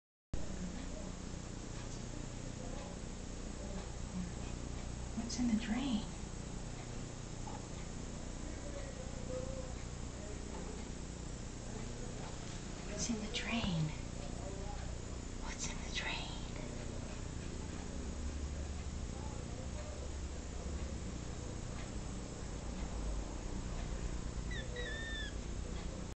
Peabody shower
My cat Peabody who is now in heaven ...liked to "chase the drain"...he is very much missed and went way too soon.